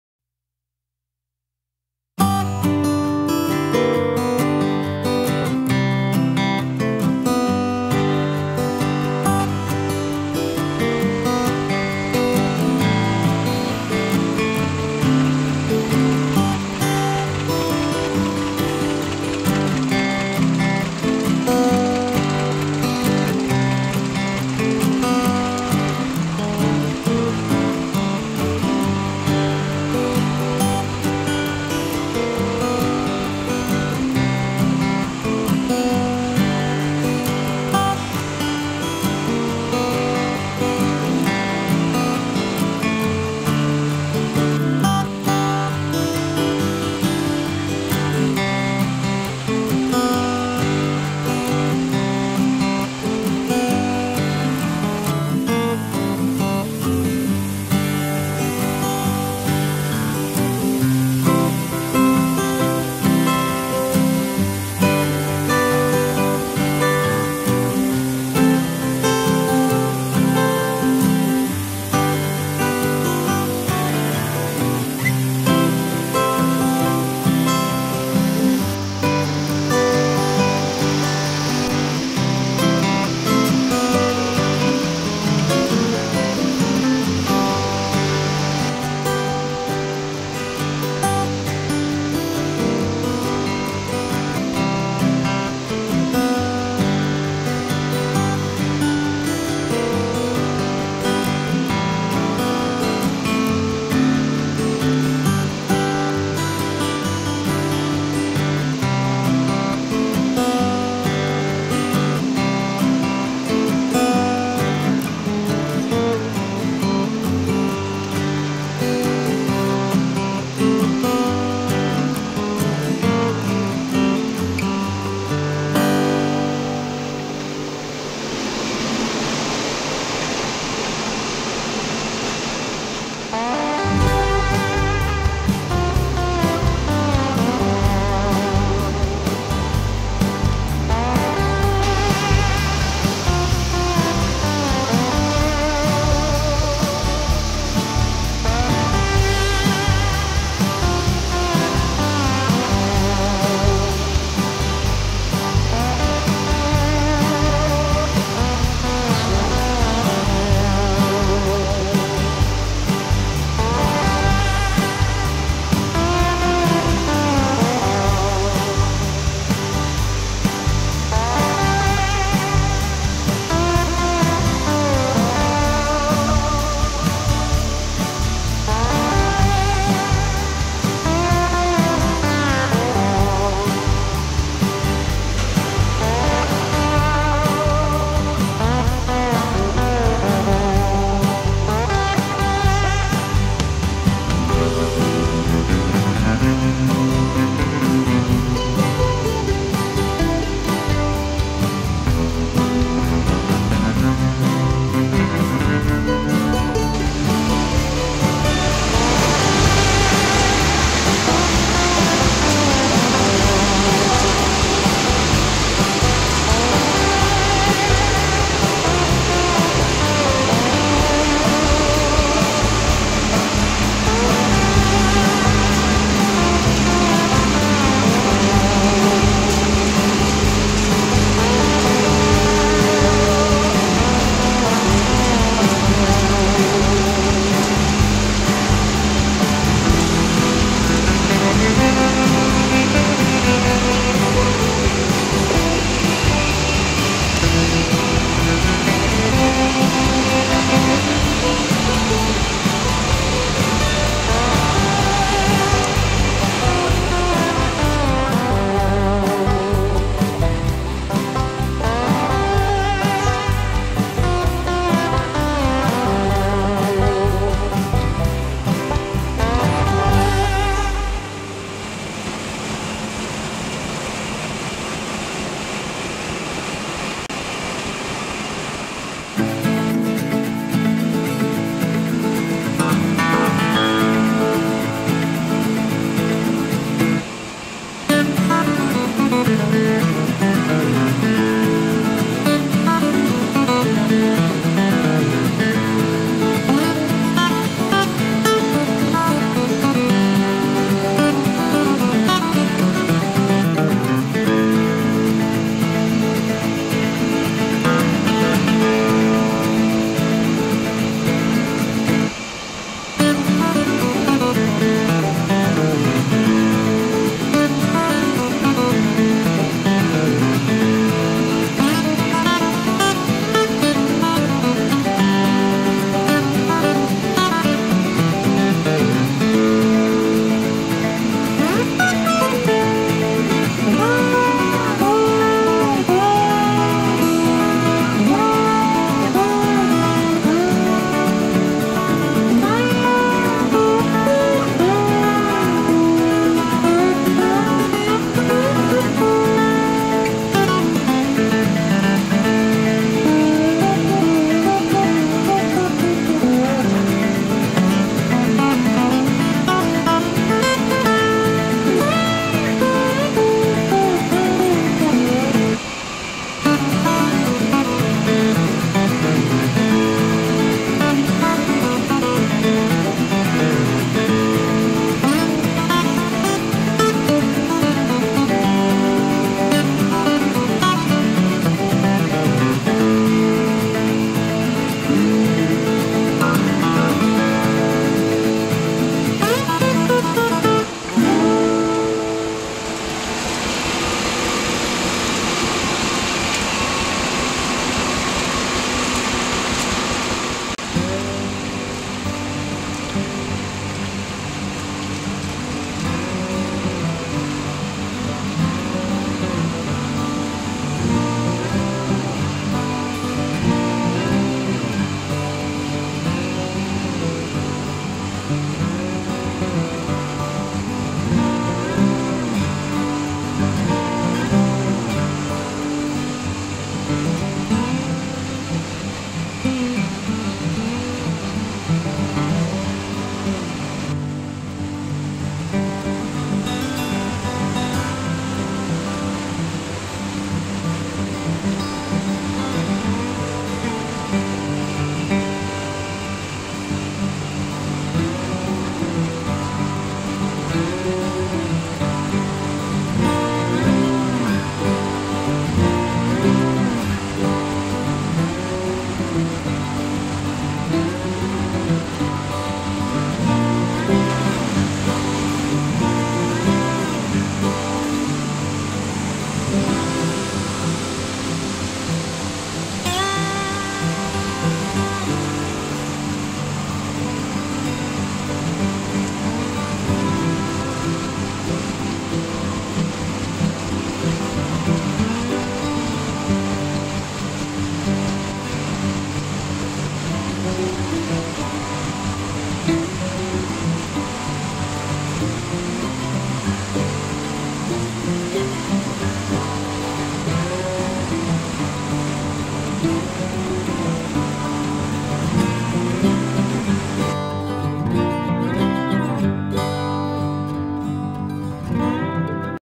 Corn Mill in Nabunturan Davao De Oro Philippines
Blackstone 24 hp corn mill in Nabunturan,Davao de Oro. we made a small sifting to clean corn kernel new invention